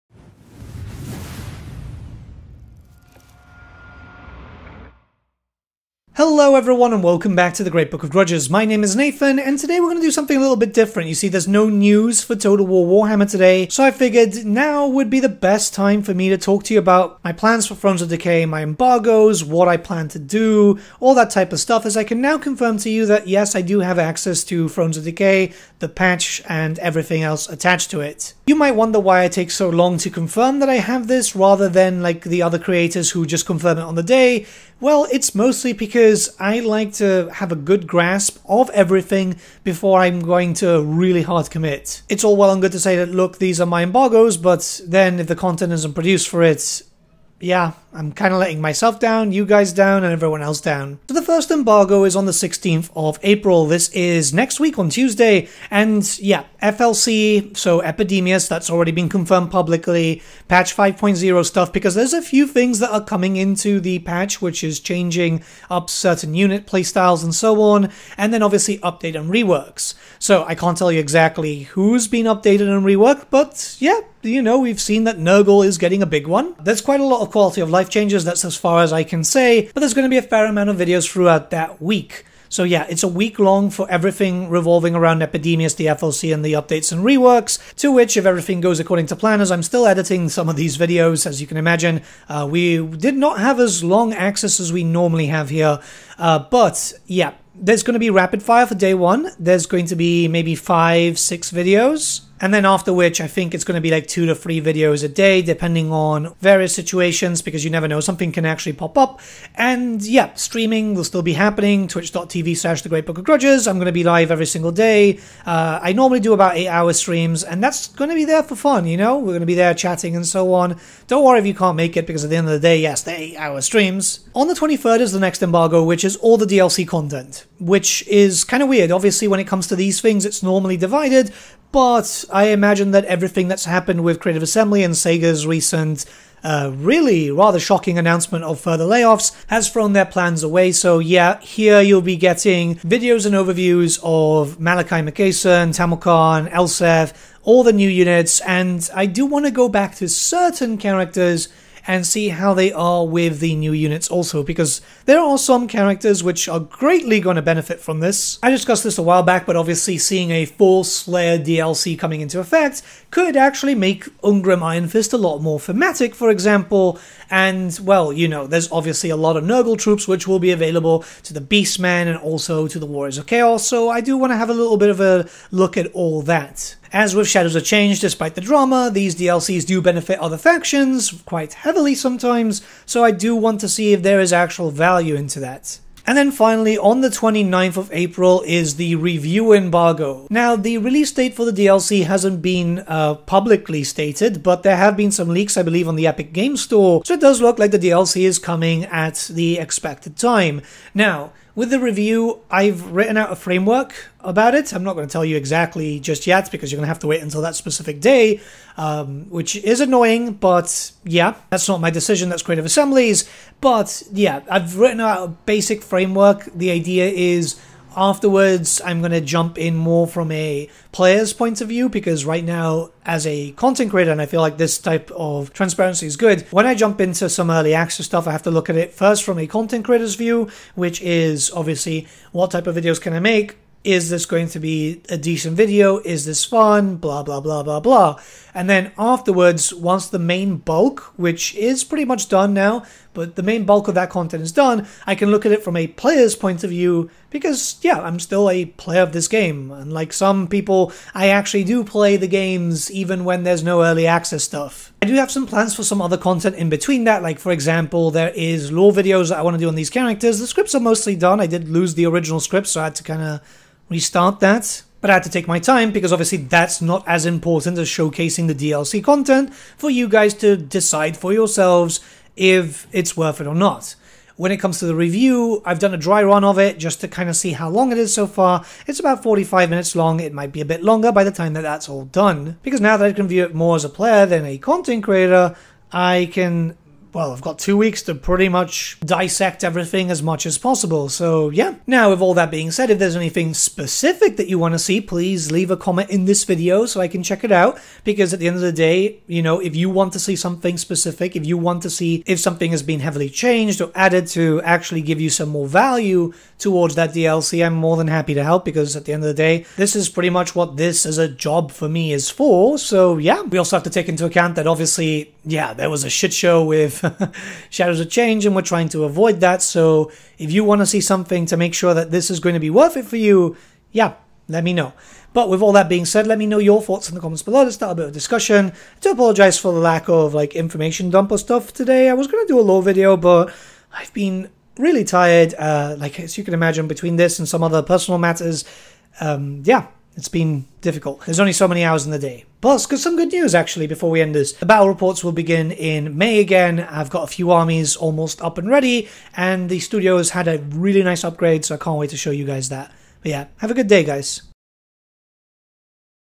My Plans & Embargo Details For THRONES OF DECAY - Total War Warhammer 3
I can now confirm I have early access to Thrones of Decay so today I wanna go over some plans I have of this brand new DLC and when exactly I'll be dropping some early access content for the sake of transparency with you guys. Let's chat :)

Special thanks to our Patrons:

Fabi, faithlessberserker, Gibraltar LUSC, Ryan Bertsche, Mike7676, Monkeyman, VIKRAM SRINIVASAN, TheSaltyCanadian, Valk, Rowan, Naldor, BearlyTactical

THE3476